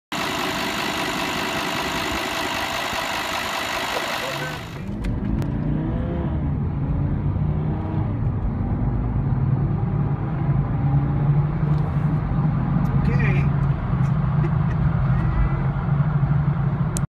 VW Touareg V10 TDI 0-70 MPH acceleration
Test driving a very nice 2004 V10.